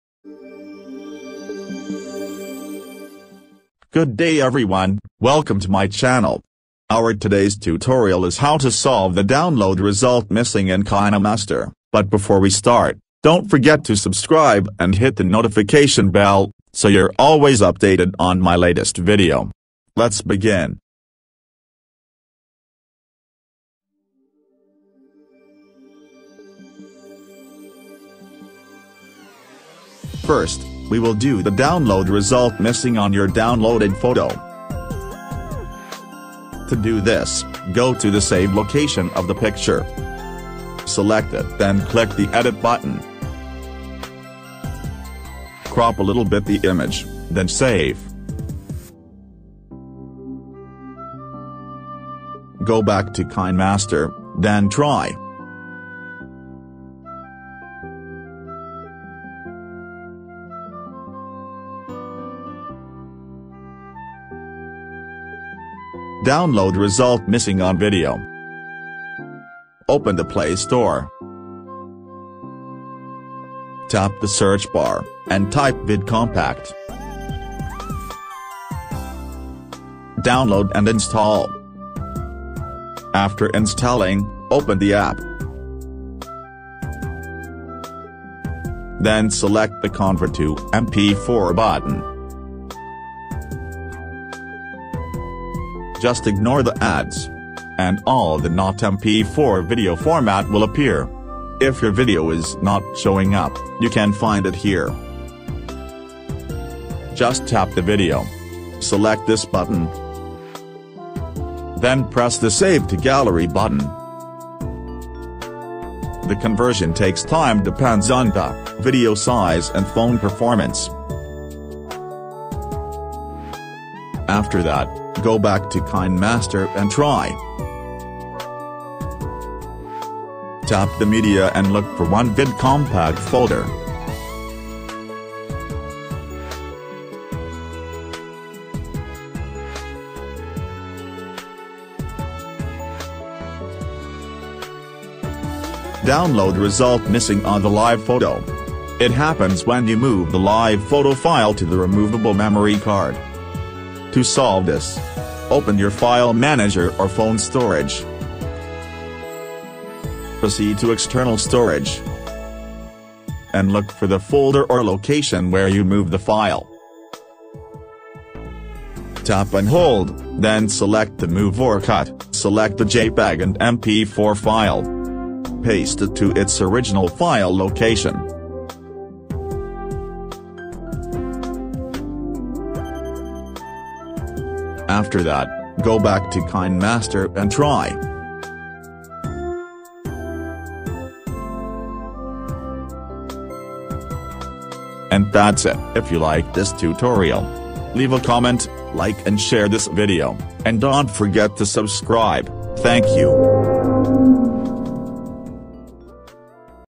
Download Result Missing | Kinemaster
I hope for your help. Thanks.
Learn how to fix the Download Result Missing in KineMaster.
1. Downloaded Photo. 0:26
2. Video. 1:05
3. Live Photo. 2:24

If you have any questions or request just leave a comment on any of my videos.